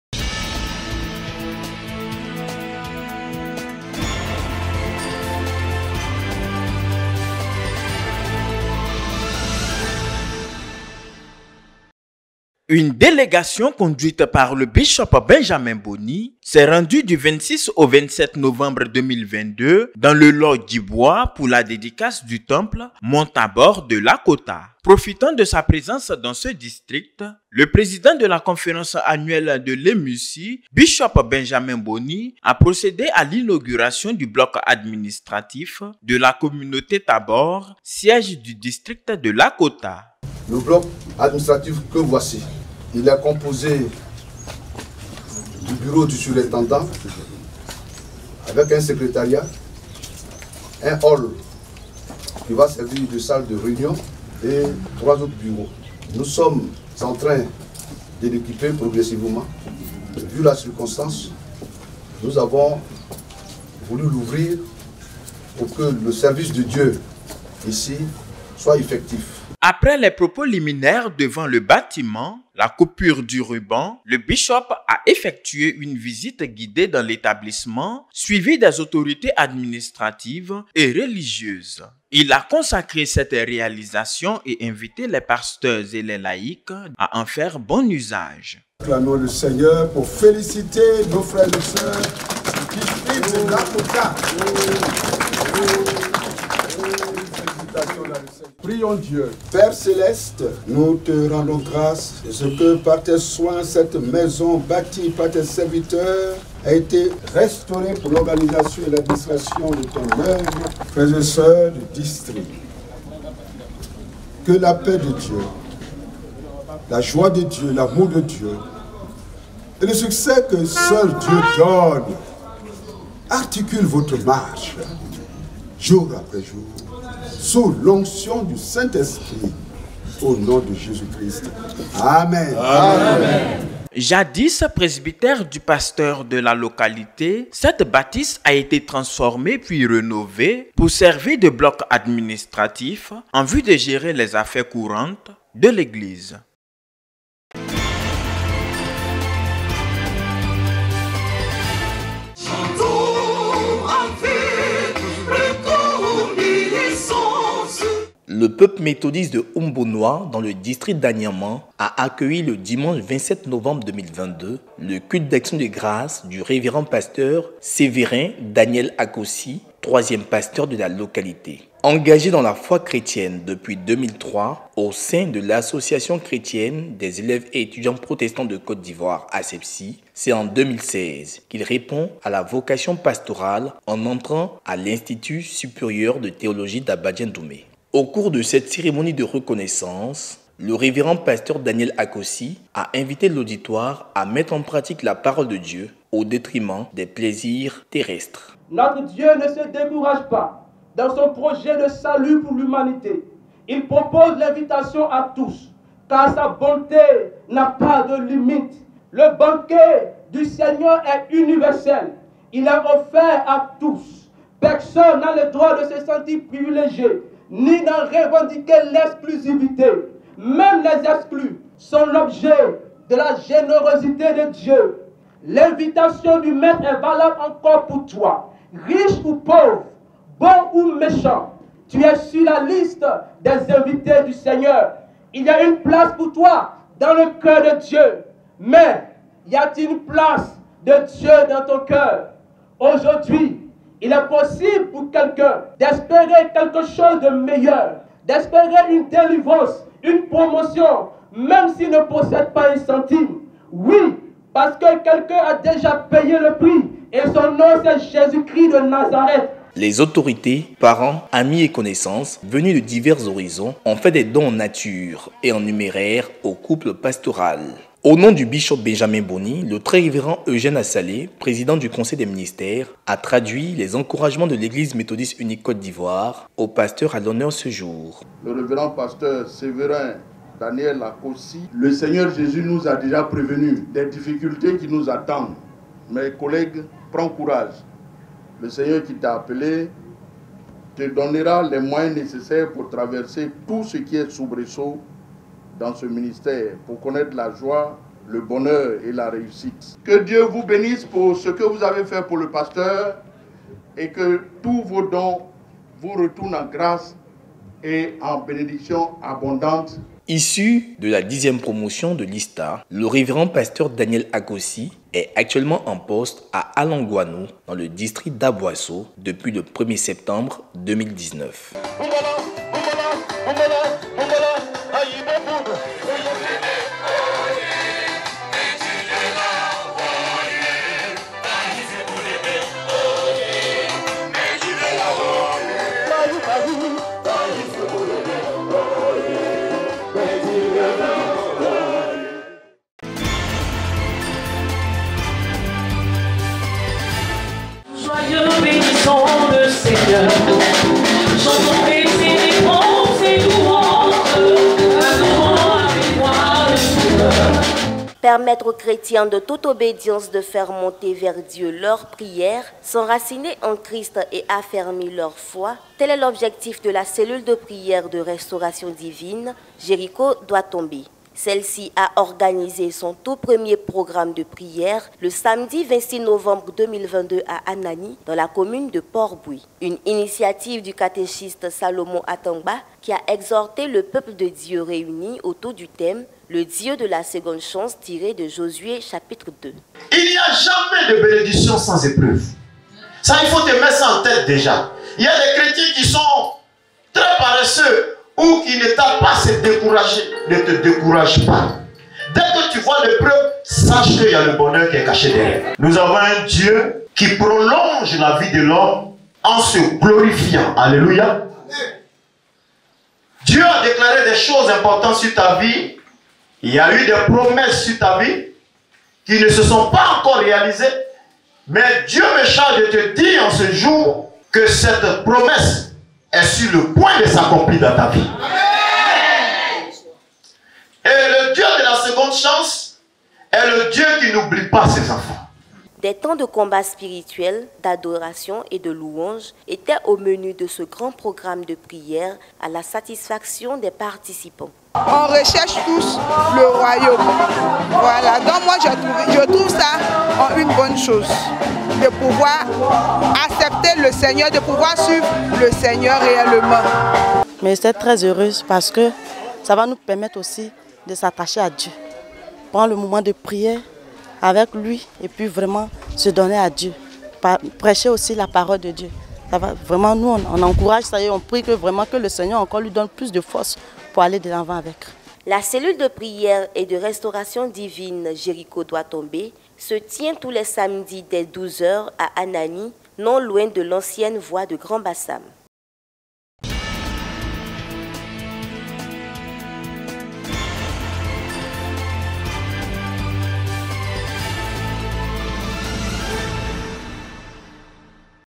FLASH D'INFORMATIONS DU 26 NOVEMBRE AU 02 DECEMBRE 2022
1. Inauguration du bloc administratif de l'EMU Mont Thabor de LAKOTA
2. Culte d'action de grâces du Révérend Pasteur Daniel AKOSSI à M'bonoua dans le District d'Anyama
3. Ouverture de la cellule de Prière et restauration "Jéricho doit Tomber" à Anani dans la commune de Port-Bouët